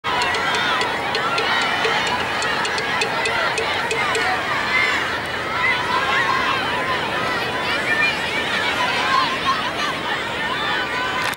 Mudtex2015 Samiya Loses a Cleat
A classic "Mudtex moment" from Sunday morning at 2015 Women's Centex as Samiya loses a cleat in the muck and has to take a sub.